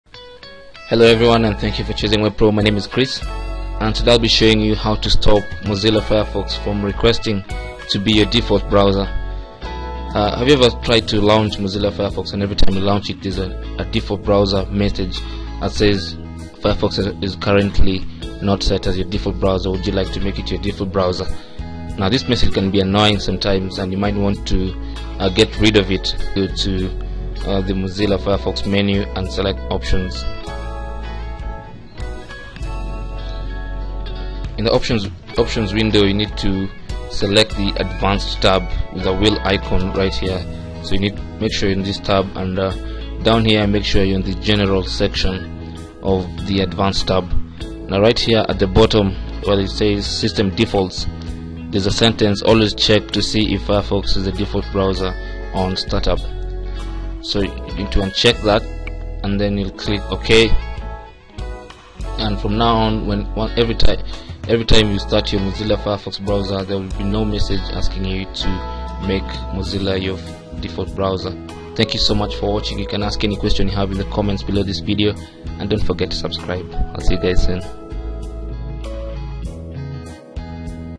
How To Stop Firefox Default Browser Message
Does Your firefox browser keep requesting you to make it its default browser, you can stop it from interrupting your work with that nagging question. Just follow the steps in the video.